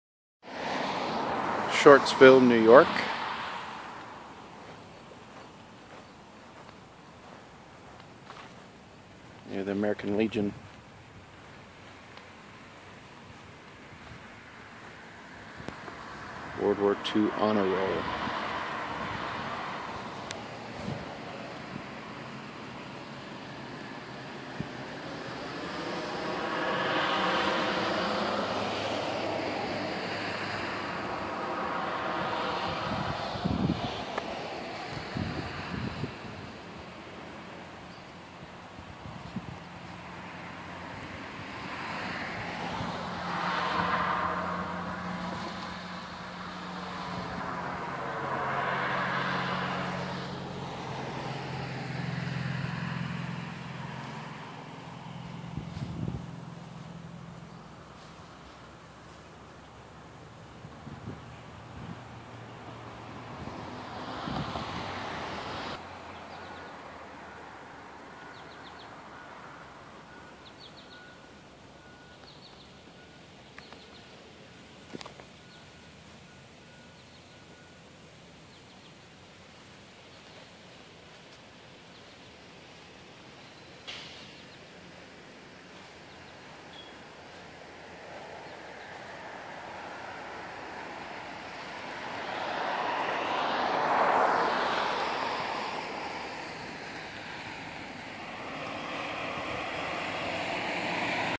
WWII Honor Roll Shortsville New York
Nice momument to those who serviced and those who paid the ultimate price during World War II from Shortsville,  NY.